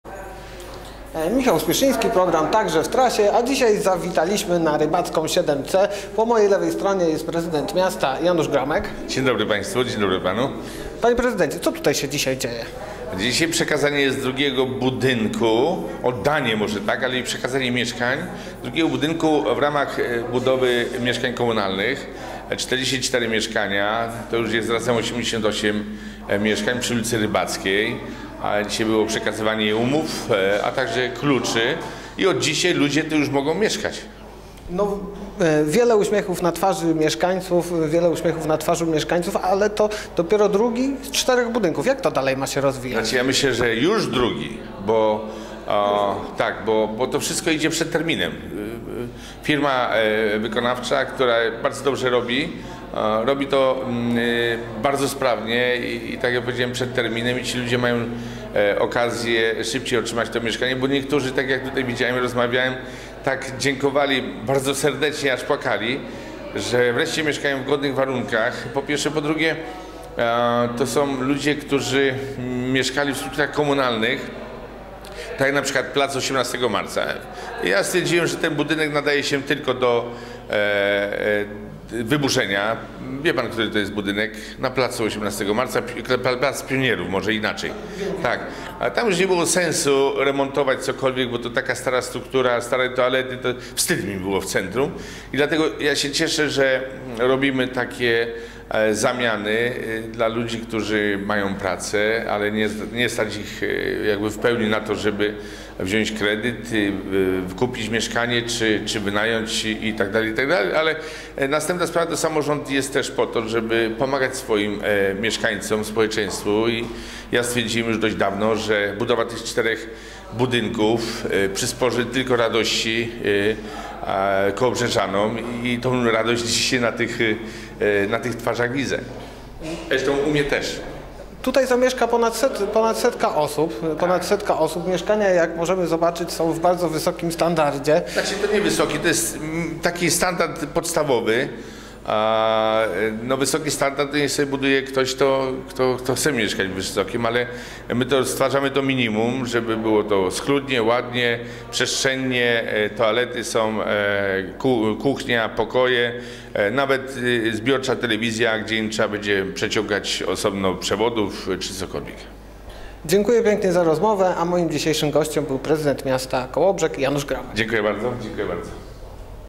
44 nowe mieszkania - TAKŻE W TRASIE
Naszym gościem był Prezydent Miasta Kołobrzeg Janusz Gromek